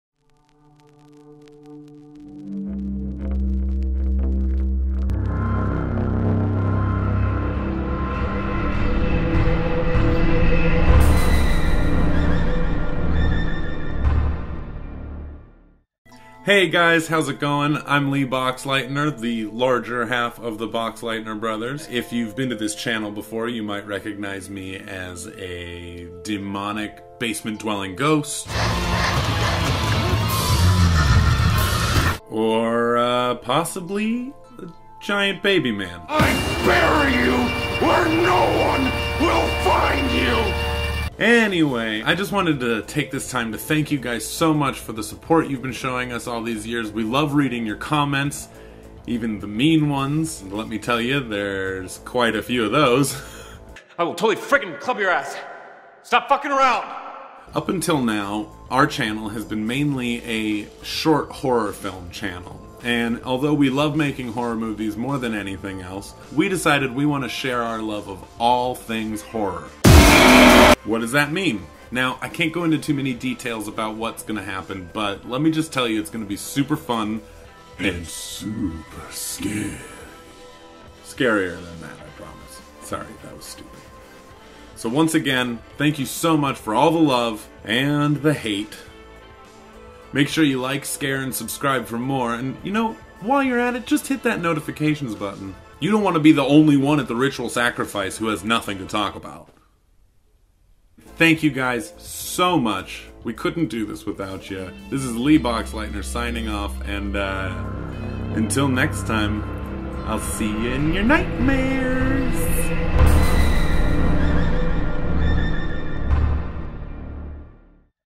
THANK YOU for being a fiend!!!
We just wanted to make this video to say THANK YOU to all of our subscribers and supporters.  From now on, we are going to bring you MORE than just horror shorts!